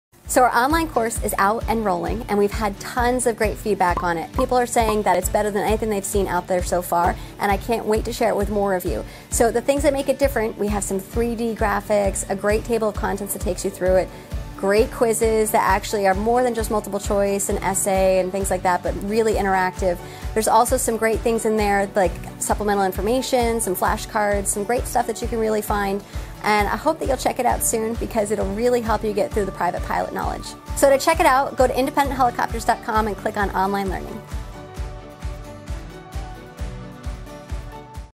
Private Pilot Online Training Course with Independent Helicopters
Our online courses are designed to be effective and to help you reach the correct level of understanding in the comfort of your own home or office.